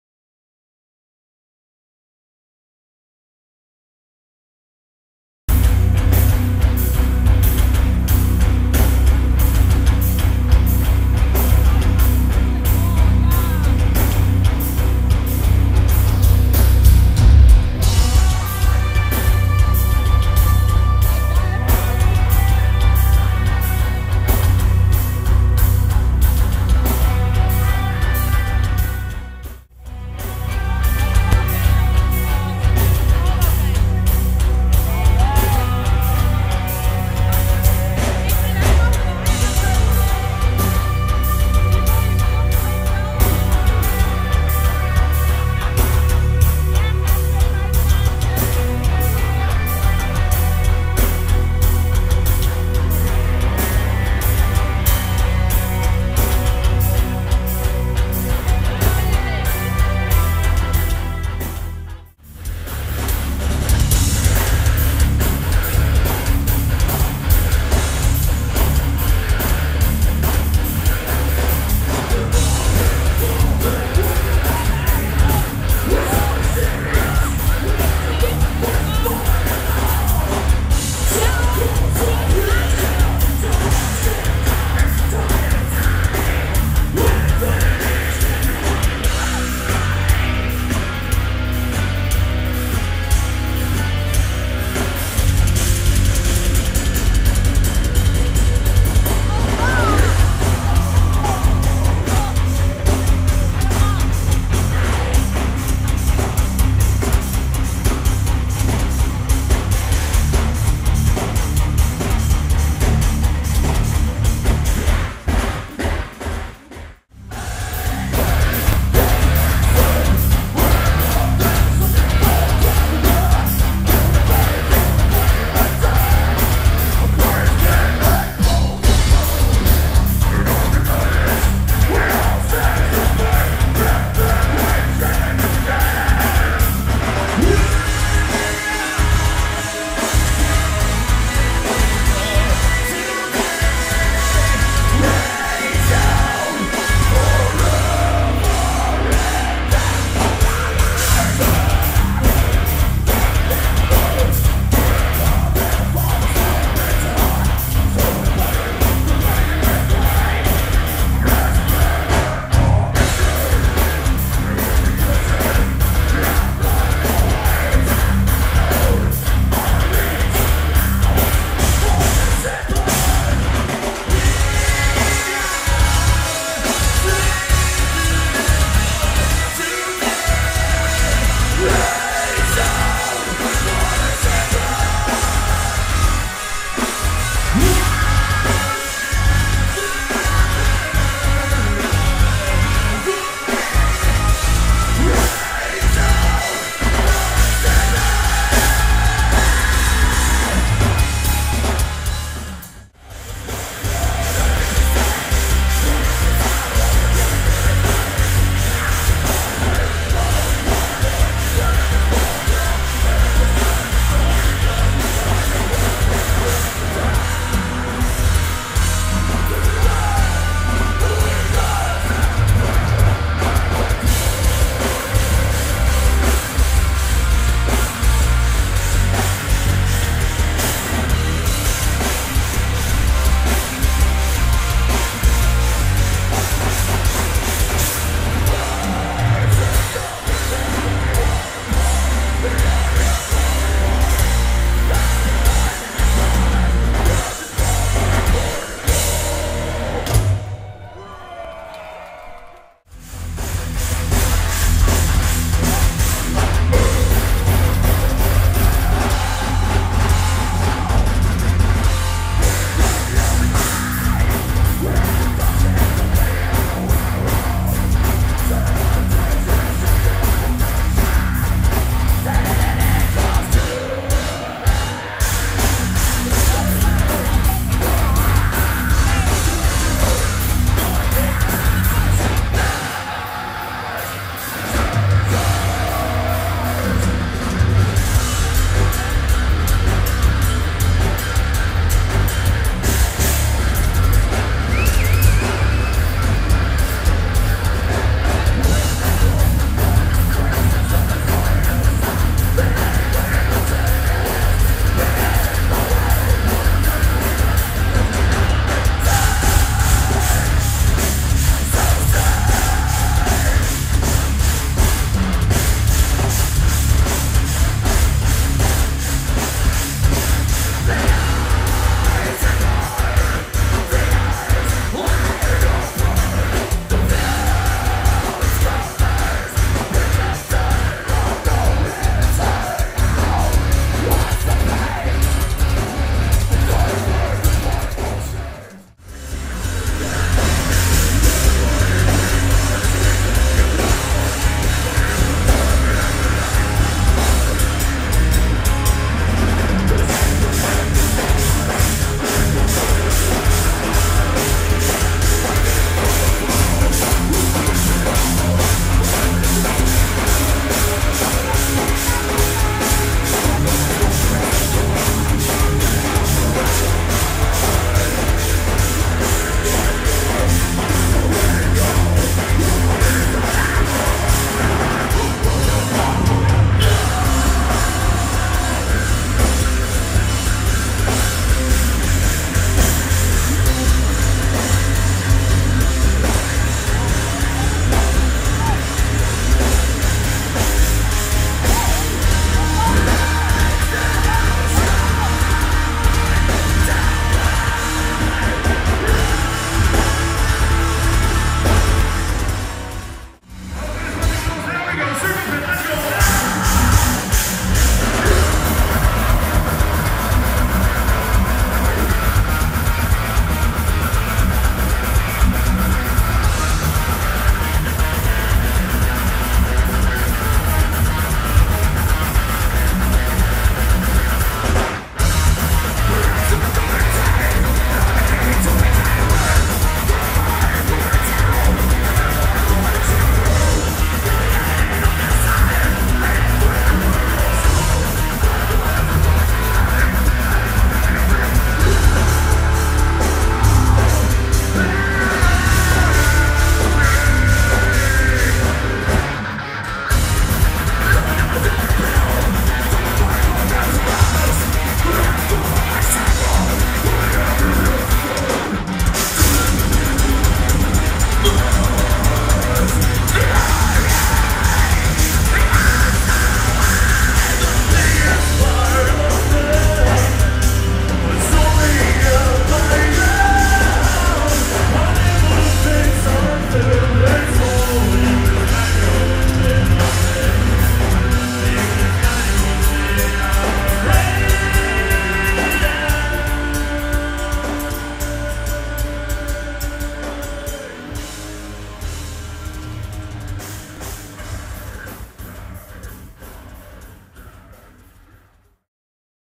Textures - Compilatie Video @ Zwarte Cross
Textures is a metal band from Tilburg, the Netherlands, formed in 2001. The band plays a complex metal style, closely linked with experimental/progressive metal (and bands like Meshuggah), featuring elements of death metal and thrash metal; they also incorporate melodic interludes into their songs.

This is recorded at the Zwarte Cross 2012 on Saturday 21th of July 2012 in Lichtenvoorde, the Netherlands.

Daniel de Jongh -- vocals
Jochem Jacobs -- guitars
Bart Hennephof -- guitars
Stef Broks -- drums
Remko Tielemans -- bass
Uri Dijk -- synths